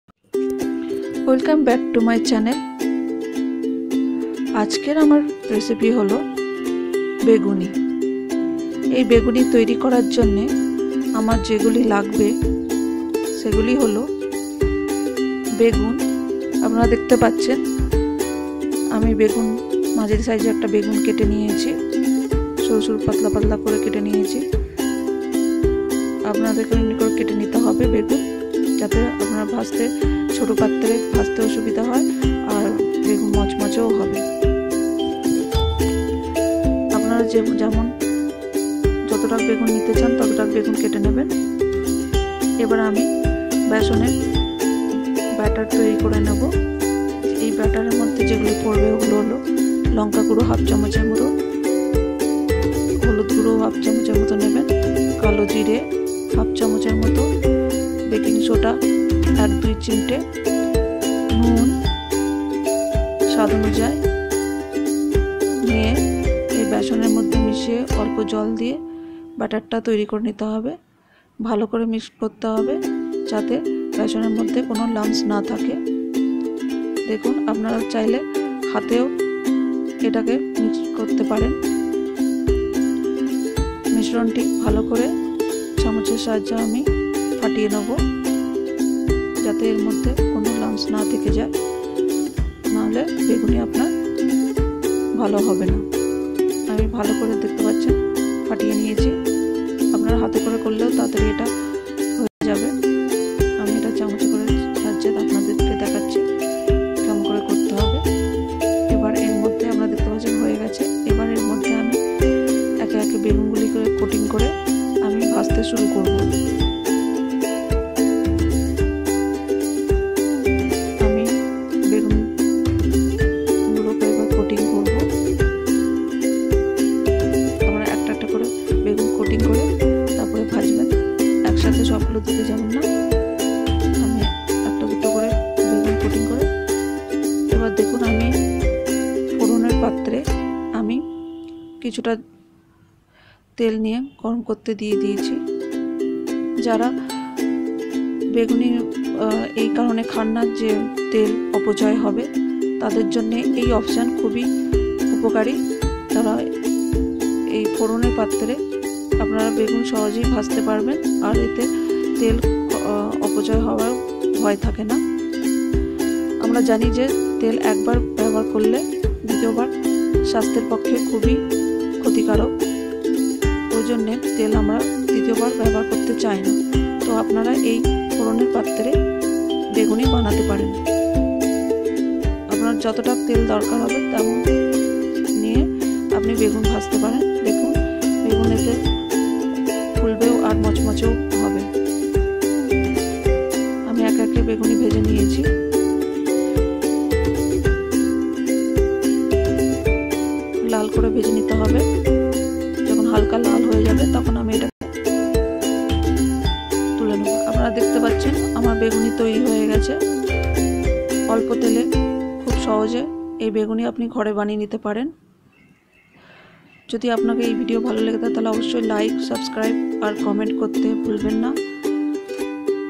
অল্প তেলে মুচমুচে পারফেক্ট ইফতার স্পেশাল বেগুনি রেসিপি ||Perfect Crispy Beguni recipe
ASMINA'S KITCHEN & VLOG, Asmina's kitchen & vlog,অল্প তেলে মুচমুচে পারফেক্ট ইফতার স্পেশাল বেগুনি রেসিপি ||Perfect Crispy Beguni recipe